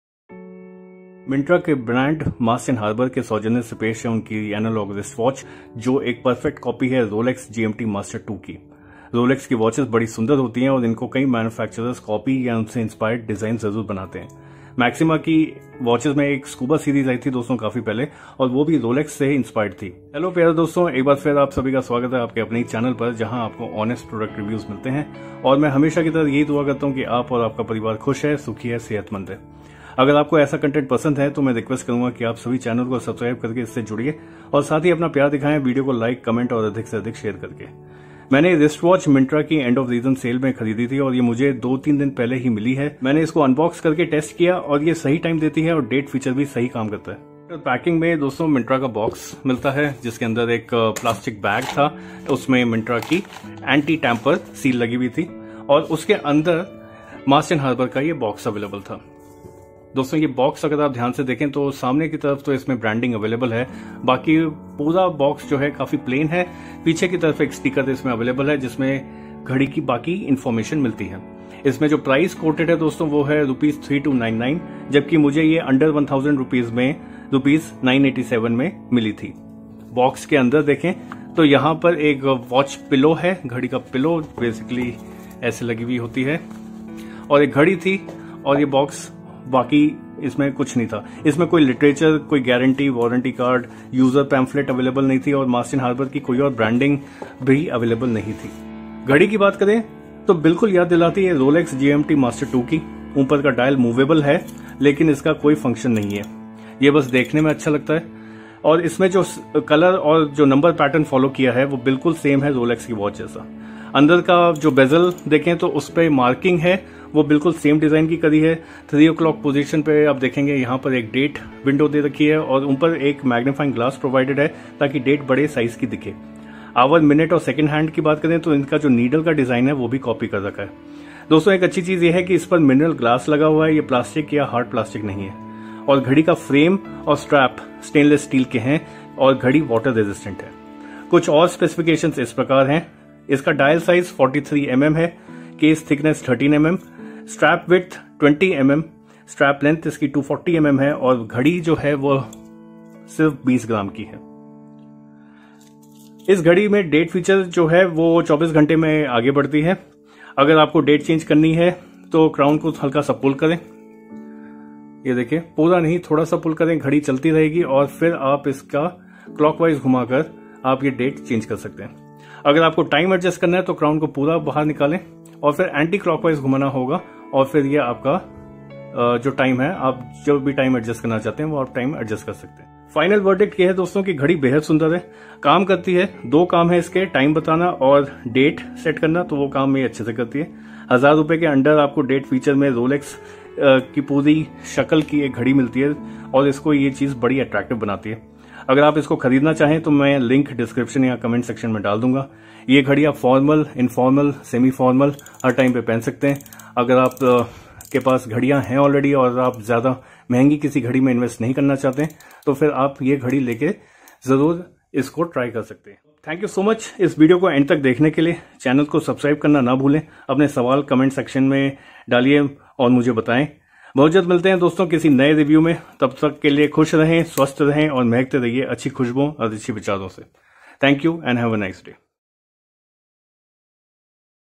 Mast & Harbour Rolex Look-alike Analog Watch | Myntra EORS special
Hello doston,

swagat hai aap sabhi ka ek baar fir aapke apne hee channel par.

iss review me janiye Myntra ki under ₹999/- watch ke baare me.

ye ghadi ki looks inspired hain Rolex GMT Master 2 se aur bilkul bhi cheap nahi lagti.

aap iss ghadi ko formal / semi-formal / informal occasions me pehen sakte hain.

iss ghadi se related aapke man me jo bhi sawal ho, comments section me mujhe likh bhejiye.

Purchase link:

Mast & Harbour Men Blue Dial & Silver Toned Straps Analogue Watch- MFB-PN-W4036G on Myntra